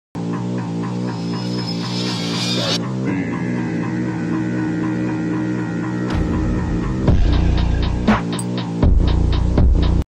The Art of Patience: Letting Truth Unfold! ⏳🔍
Sometimes, overthinking can cloud our understanding. This video explores the importance of patience in allowing the truth to reveal itself over time. Embrace the journey of discovery and trust in the process.